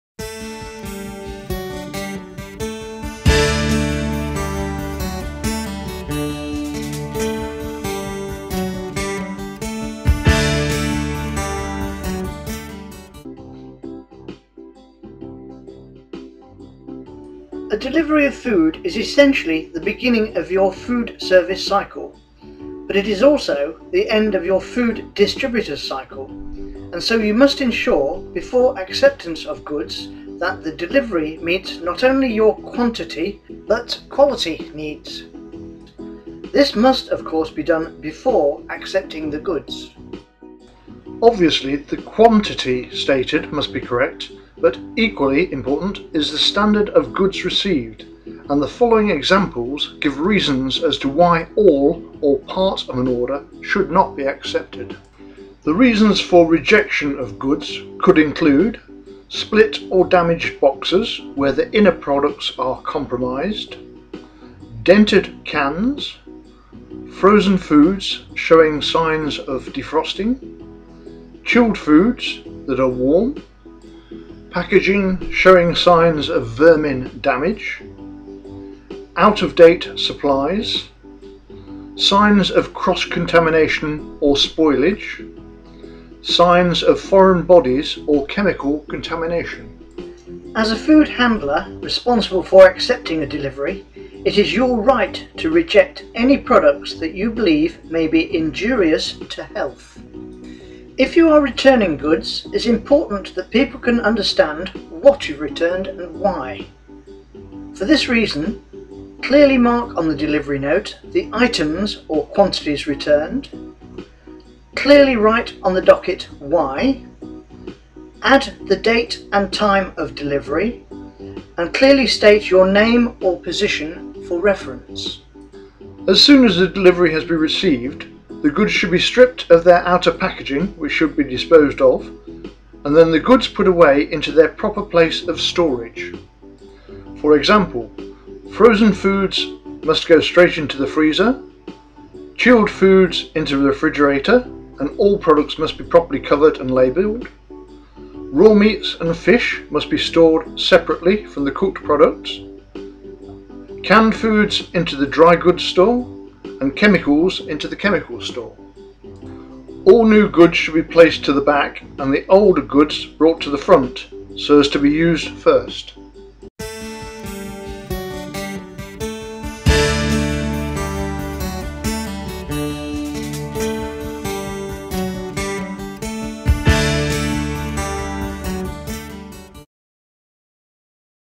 Safefood5
Food safety Certificate video 5, part of the training course to be found Level 2 course has 11 3minute max videos of training followed by one 40 multichoice quiz style exam. The approved course is created by Torchlight Training and the final self print certificate is a SureServe registered